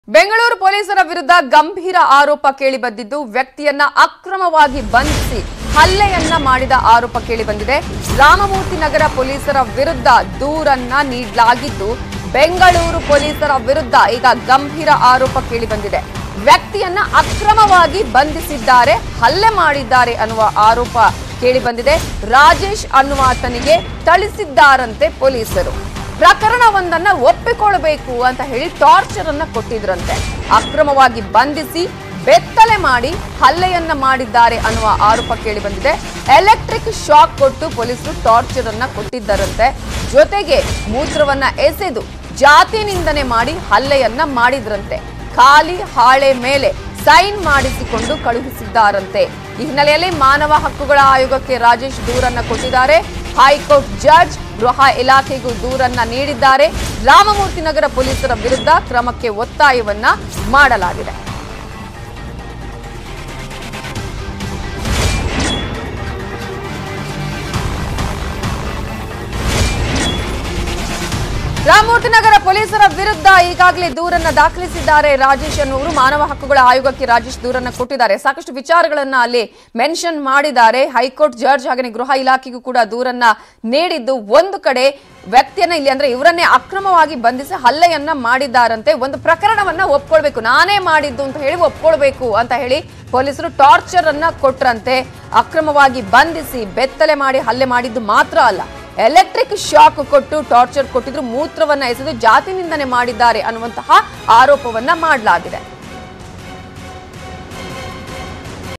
Bengaluru | Ramamurthy Nagar Police ವಿರುದ್ದ ಆರೋಪ; ಠಾಣೆಯಲ್ಲಿ ಅಕ್ರಮವಾಗಿ ಬಂಧಿಸಿ ಮನಸ್ಸೋಯಿಚ್ಛೆ ಹಲ್ಲೆ!
Bengaluru | Ramamurthy Nagar Police ವಿರುದ್ದ ಆರೋಪ; ಠಾಣೆಯಲ್ಲಿ ಅಕ್ರಮವಾಗಿ ಬಂಧಿಸಿ ಮನಸ್ಸೋಯಿಚ್ಛೆ ಹಲ್ಲೆ. ಪ್ರಕರಣವೊಂದನ್ನ ಒಪ್ಪಿಕೊಳ್ಳುವಂತೆ ಹಲ್ಲೆ ಮಾಡಿ ಒತ್ತಾಯ ಆರೋಪ